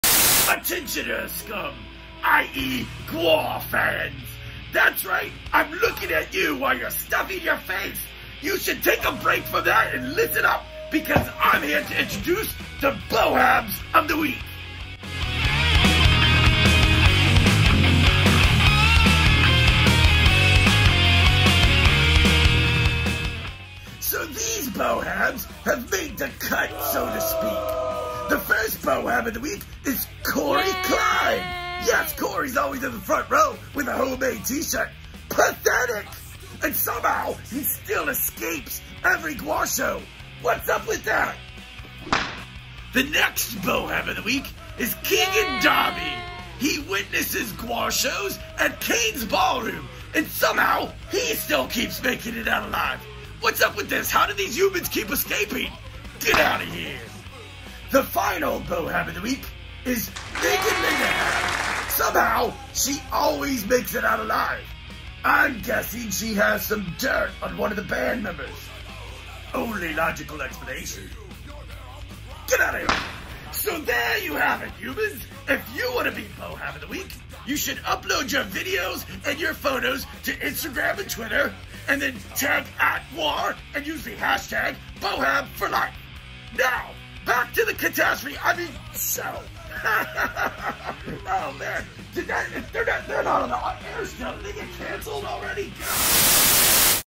Bohabs of the Week July 5, 2020
These pathetic humans were highlighted in the Live From Antarctica show on July 5th as Bohabs of the Week. You can be next! Just upload your video or photo to Twitter or Instagram, tag @GWAR and add BohabsForLife. What are you waiting for???

Watch the full Live From Antarctica episode with Pustulus, Balsac and Liam Cormier (Cancer Bats)

We command you follow the Lords and Masters on all social media!